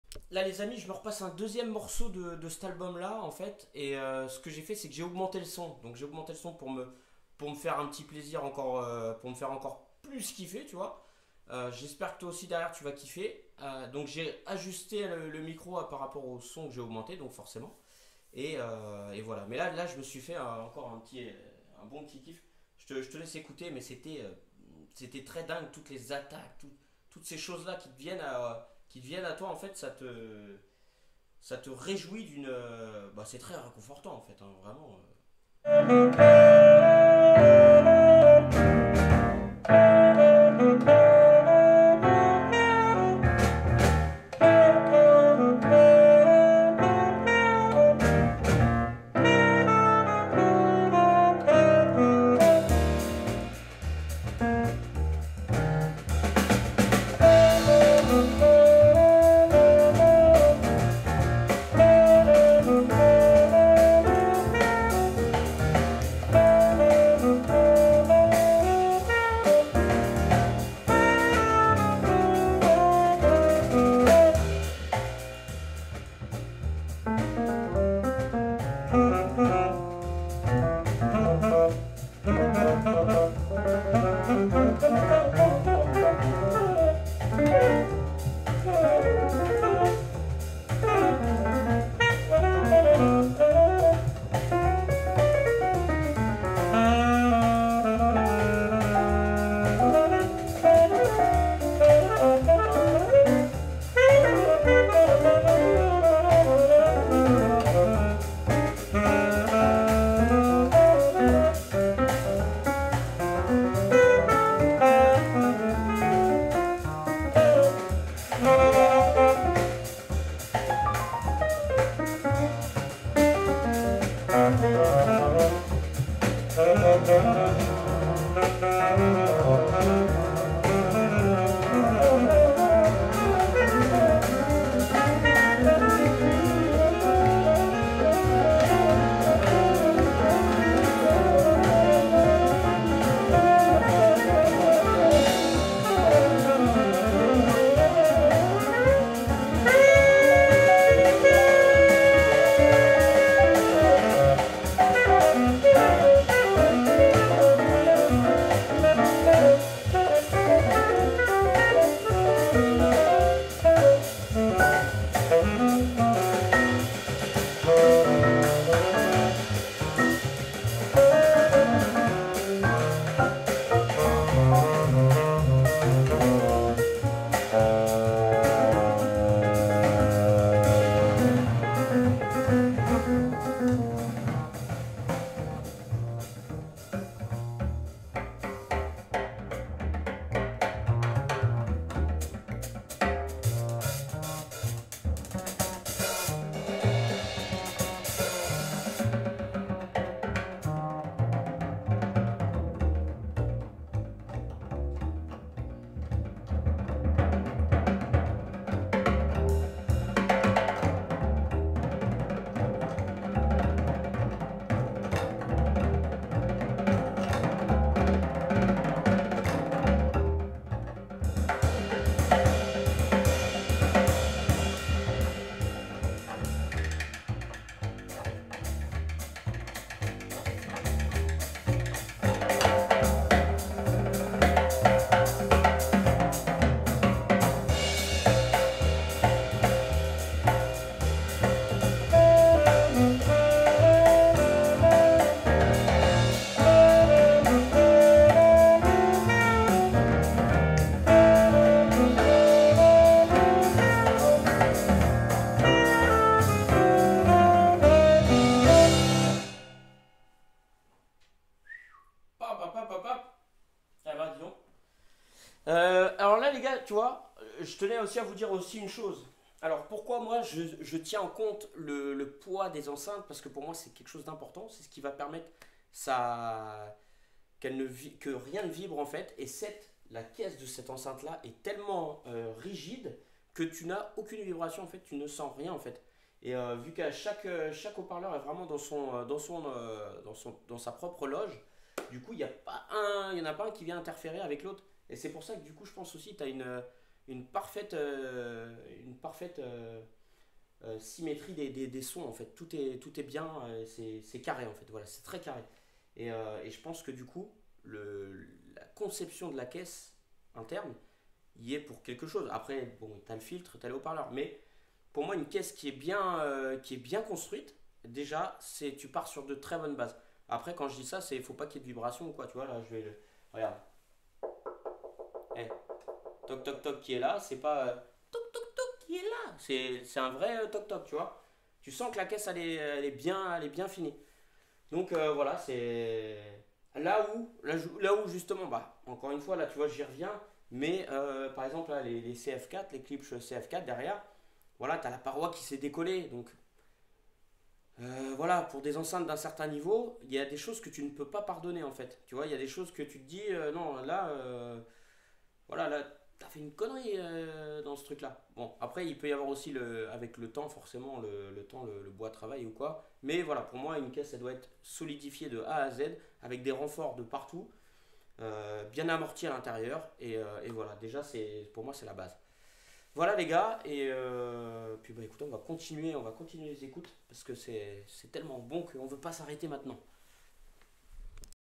Kenwood LS-1800 33kg De Muscle 💪💪💪
la caisse de l'enceinte est un element majeur gage de qualité de fabrication et surtout pour eliminer toutes vibrations interne pouvant se faire entendre à l'ecoute. j'attache de l'importance au poids de l'enceinte qui permettra de donner une inertie à celle ci une fois posée (dans l'idelal pointe de decouplage et contrepointe)
autre indicateur important si chaque haut parleur est dans sa propre loge, celà permet une mise en phase de chacun qui ne viendra pas intervertir avec l'autre afin de ne former qu'une unité d'écoute.